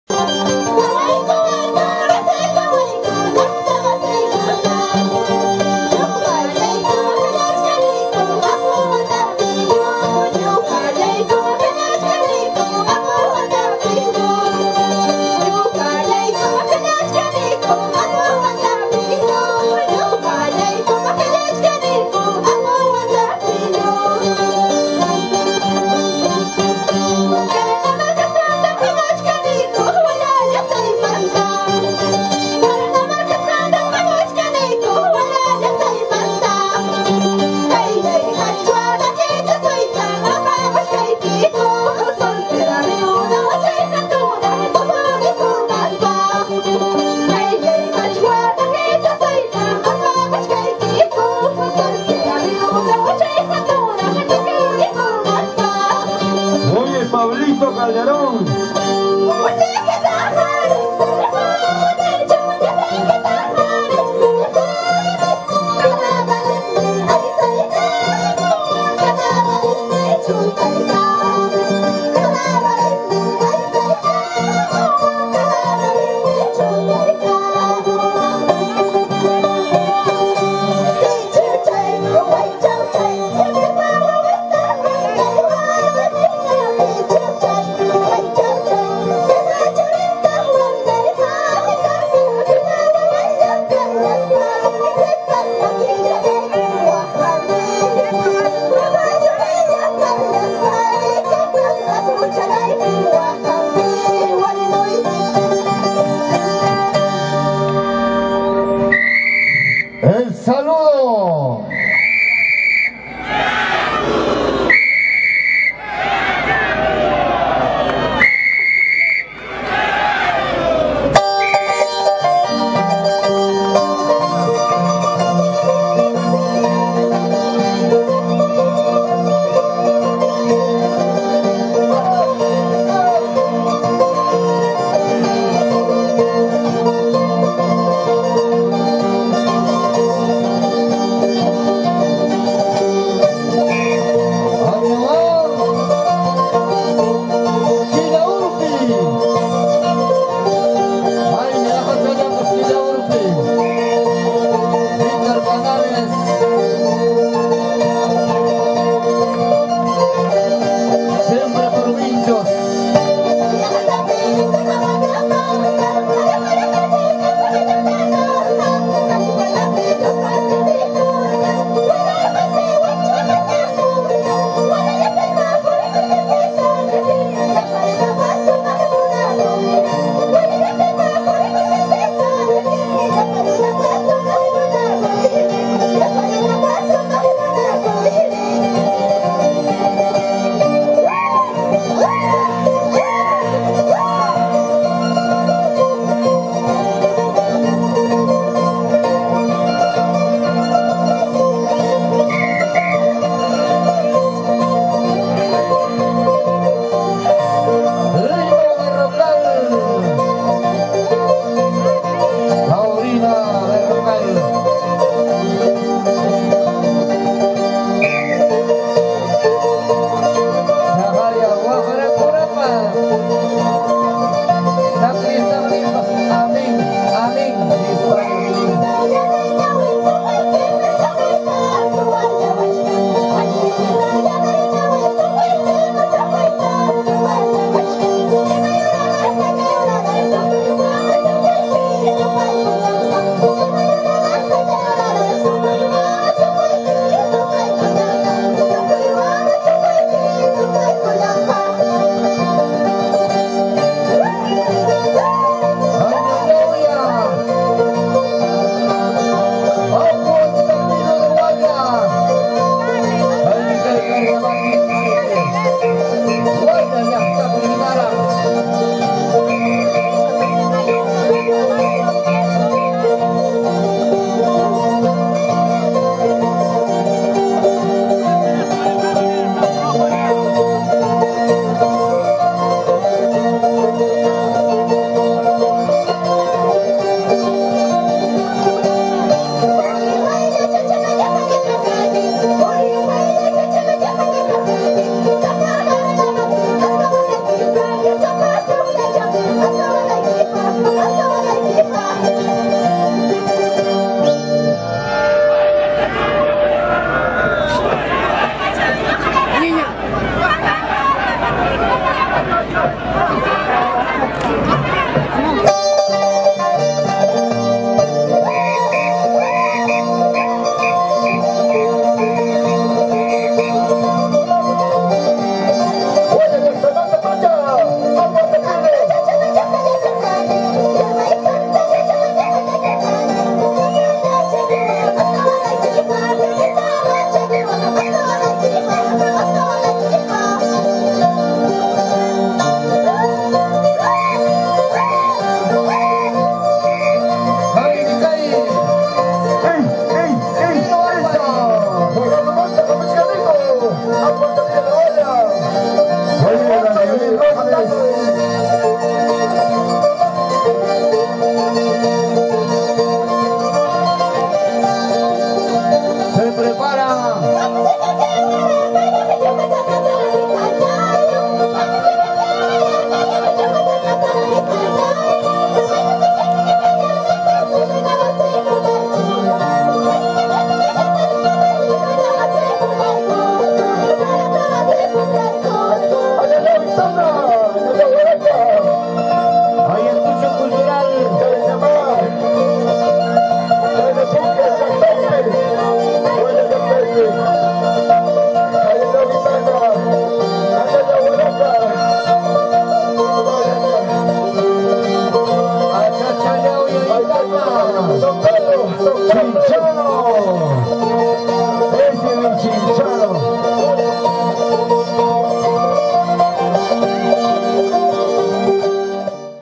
APU ANTAPILLO DE HUALLA
La presentación del Apu Antapillo de Hualla en el estadio Leoncio Prado